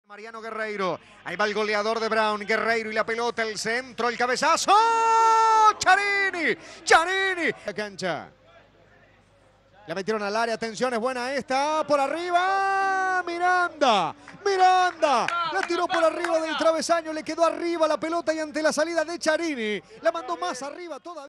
Brown tuvo las más claras. Brown (A) - Instituto. Fecha 24. Torneo Primera B Nacional
Mirá las oportunidades que tuvo Brown de Adrogué para marcar en el primer tiempo.